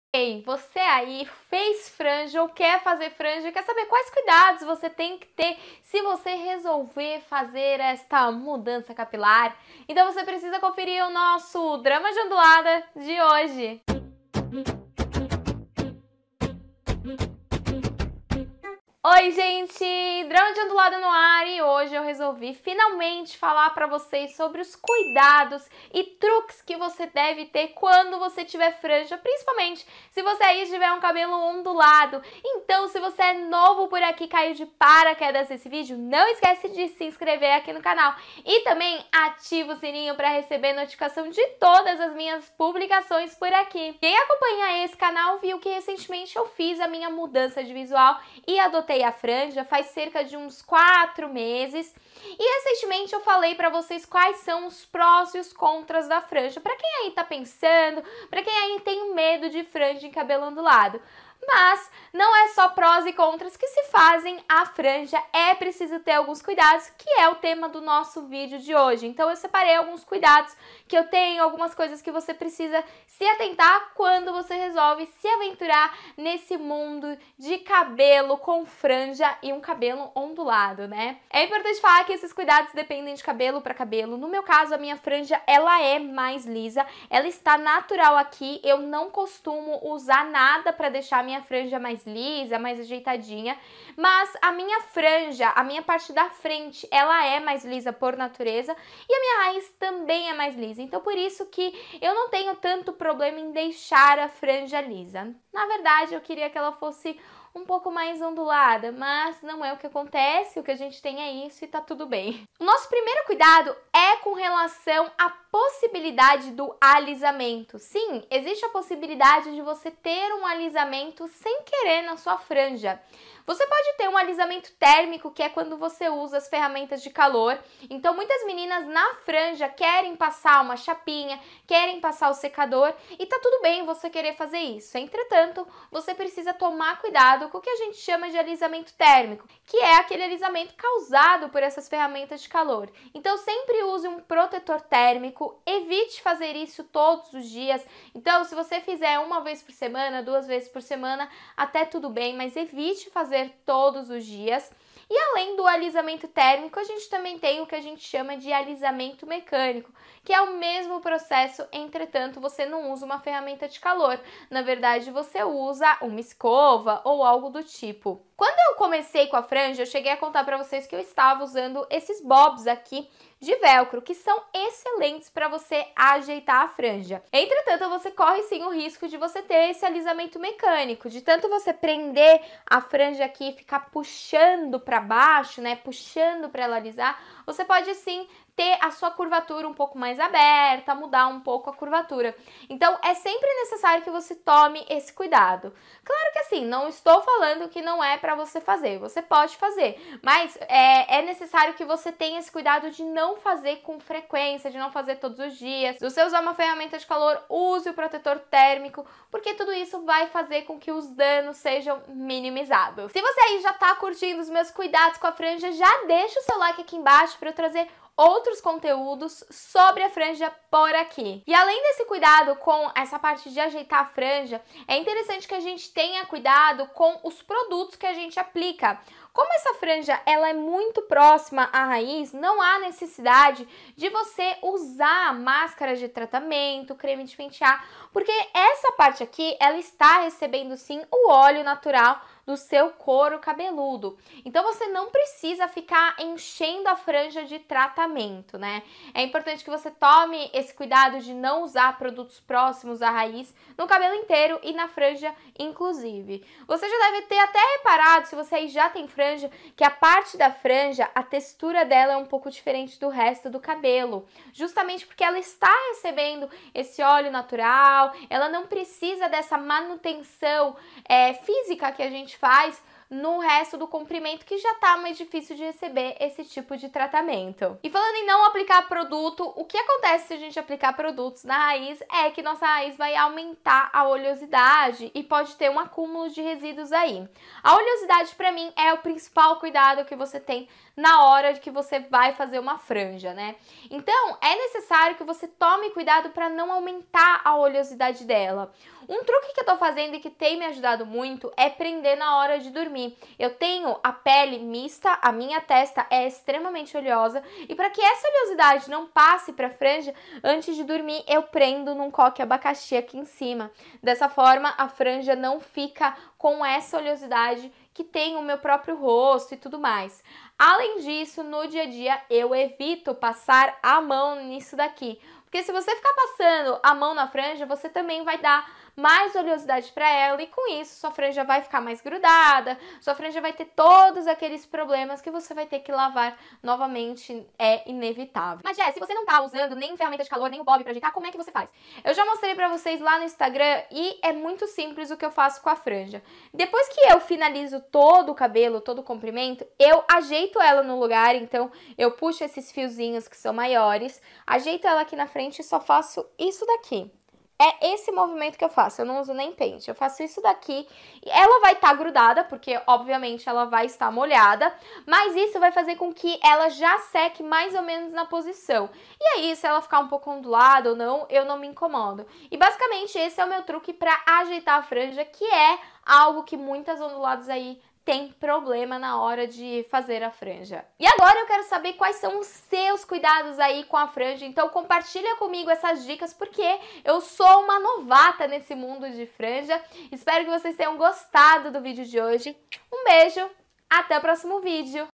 CUIDADOS com a FRANJA em cabelo ONDULADO
Oiii gente, tudo bom?

Mais um DramaDeOndulada por aqui e hoje é para falar sobre os cuidados com a franja! Como vocês já sabem, estou de franjinha já tem alguns meses e hoje resolvi contar pra vocês os cuidados que tenho com ela.

Muita gente acha que é impossível fazer franja em cabelo ondulado ou que terá muito trabalho porque precisa escovar todos os dias, mas calma! Não é bem assim.

Claro que é preciso te rum pouco mais de cuidado com algumas coisas, mas você não precisa virar escrava do cabelo só porque resolveu mudar o visual né?!